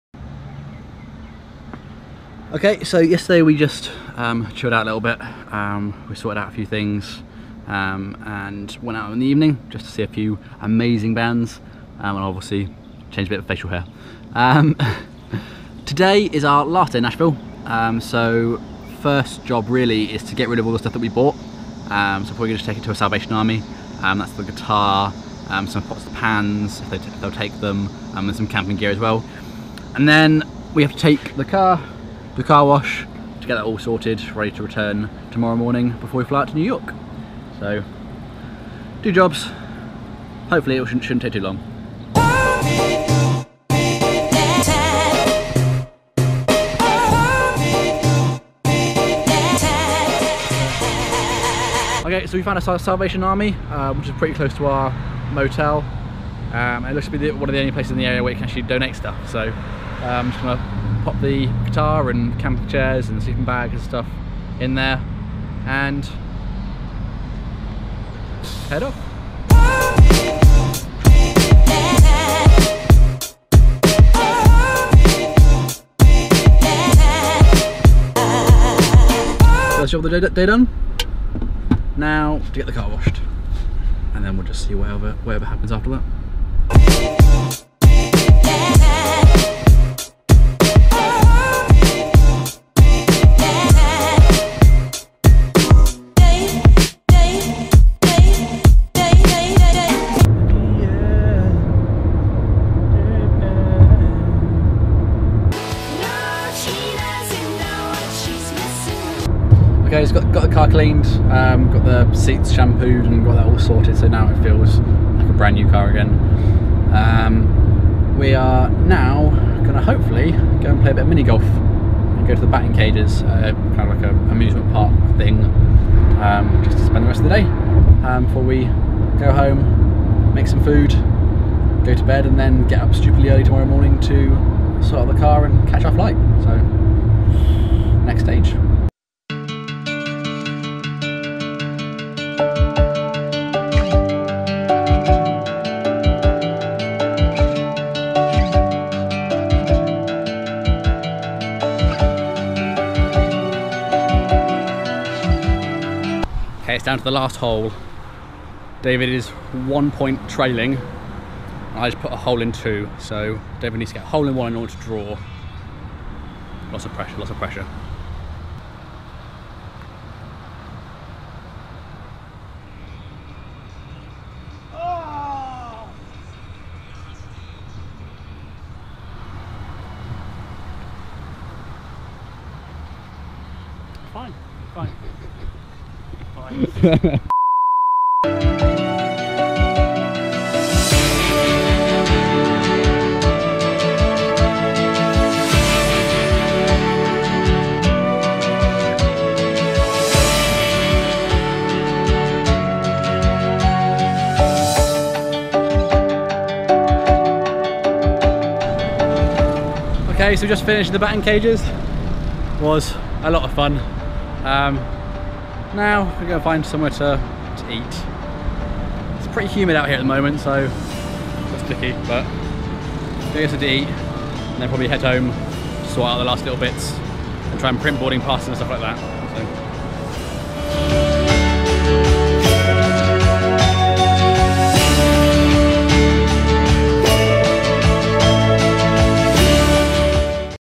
WE GAVE OUR STUFF AWAY
Today we did some odd jobs in preparation for our flight to New York.

GEAR
GoPro Hero 4
Rode VMP